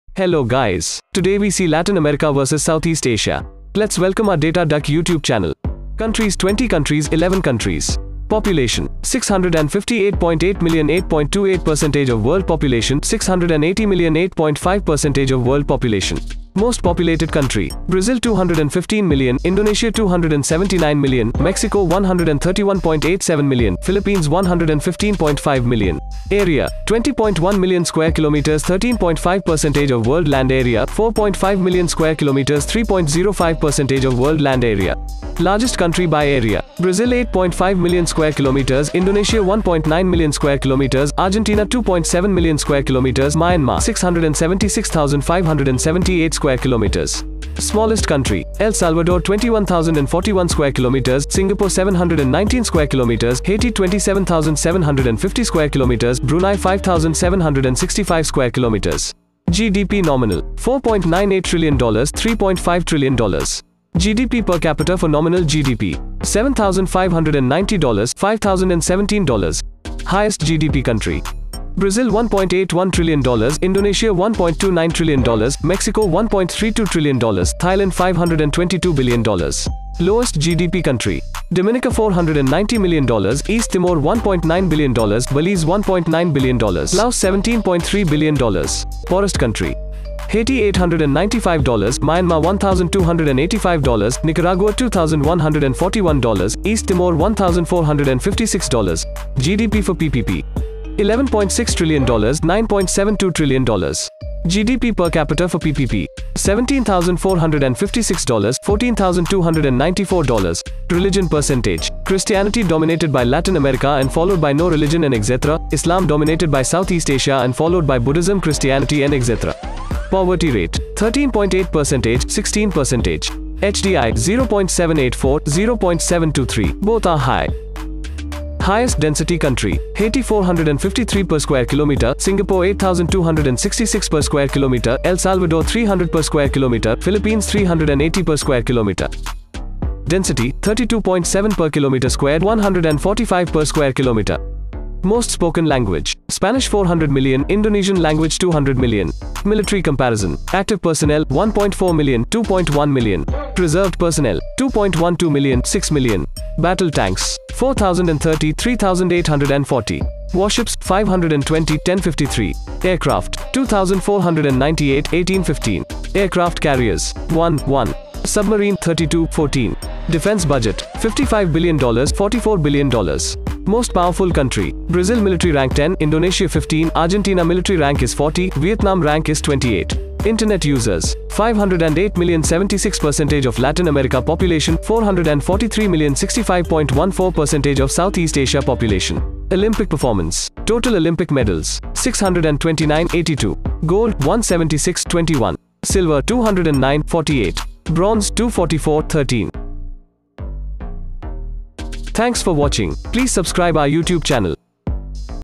Latin America vs Southeast Asia | Comparison | Data Duck
this video are Comparison between the latin America and southeast Asia.